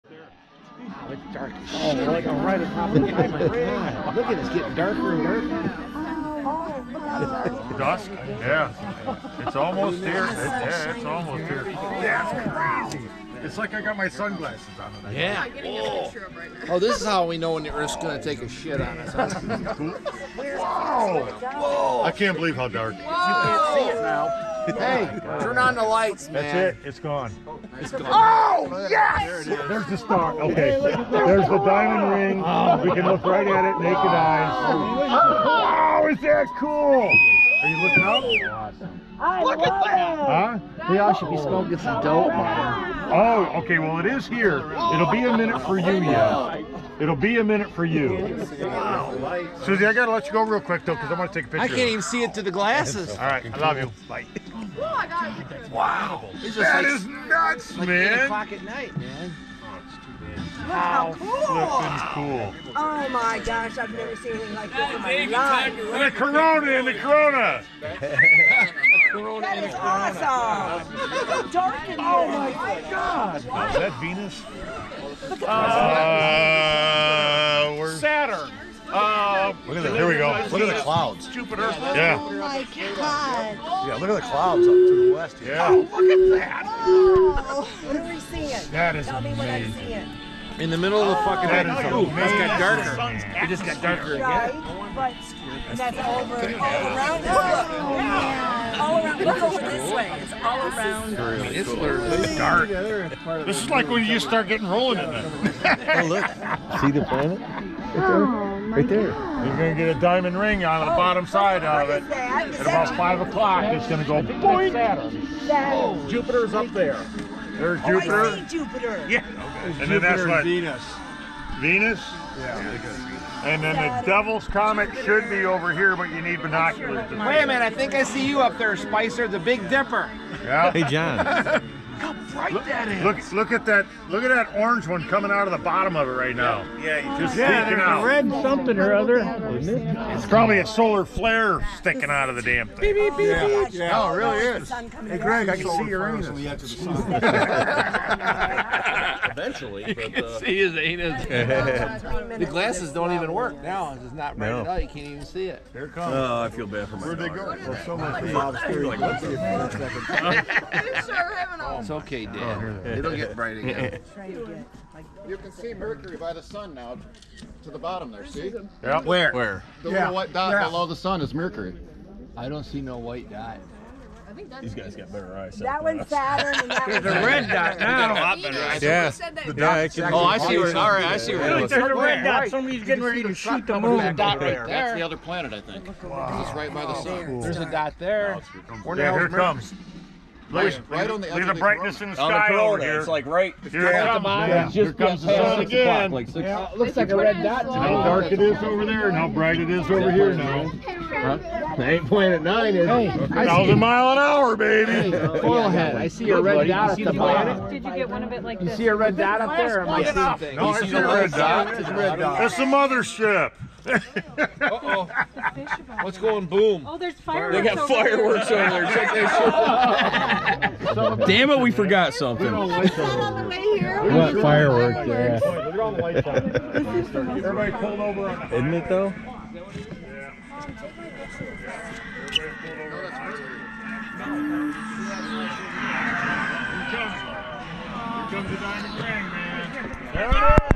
eclipse Sandusky bay 2024
Had a great spot for the 2024 eclipse on Sandusky bay with a nice south view and slightly hazy conditions. Was an amazing experience with good friends and family. Words just cant describe the event, so much bigger then i ever imagined.